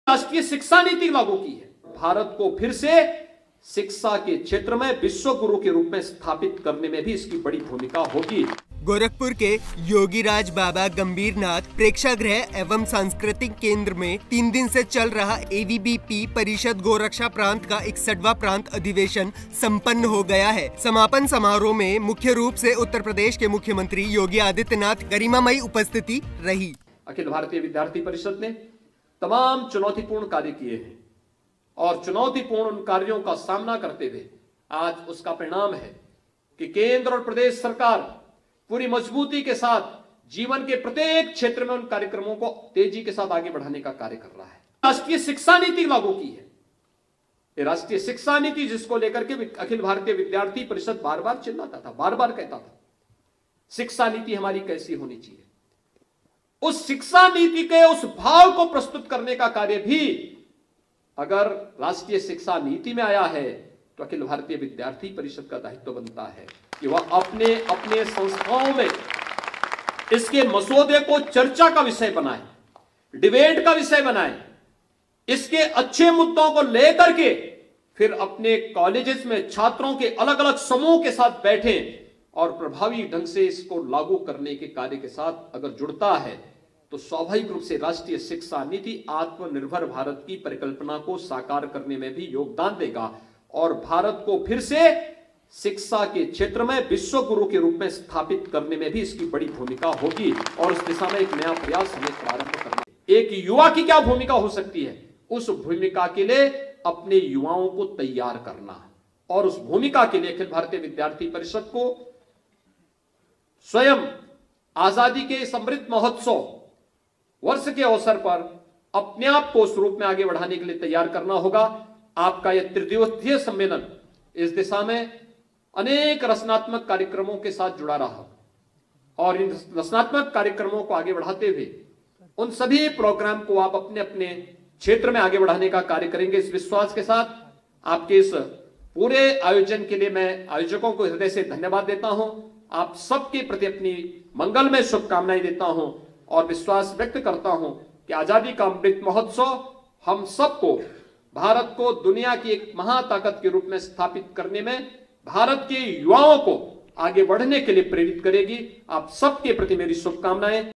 एबीवीपी परिषद गोरख प्रान्त का 61वां प्रान्त अधिवेशन संपन्न | TN15
@TheNews15 गोरखपुर के योगिराज बाबा गंभीरनाथ प्रेक्षागृह एवं सांस्कृतिक केंद्र में तीन दिन से चल रहा एबीवीपी परिषद गोरख प्रान्त का 61वां प्रान्त अधिवेशन संपन्न हो गया है। समापन कार्यक्रम में मुख्य रूप से उत्तर प्रदेश के मुख्यमंत्री योगी आदित्यनाथ की गरिमामयी उपस्थिति रही।